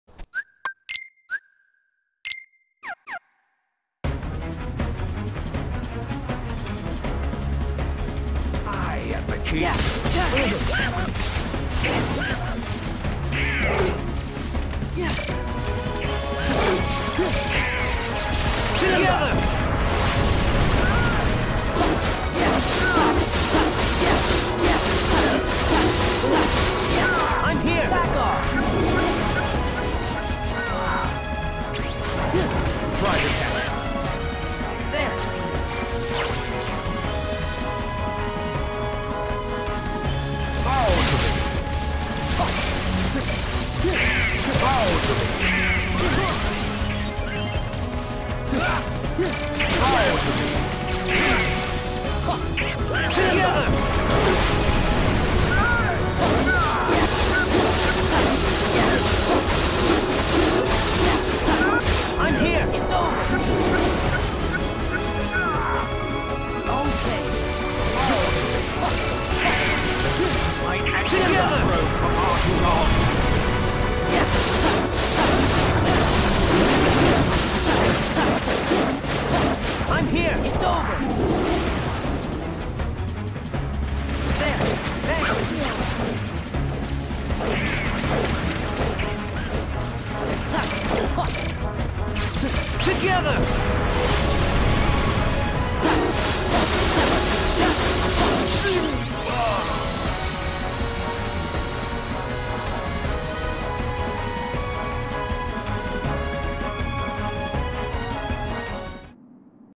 How To Chesse Your Way To Beat Scar On Level 1
My Level is 1

Equipment: Circle of Life

2 Elixirs 1 megaelixirs

Maigc: Cure, Reflect

Trick is to use wildcat before he attacks and then use a elixir before his DM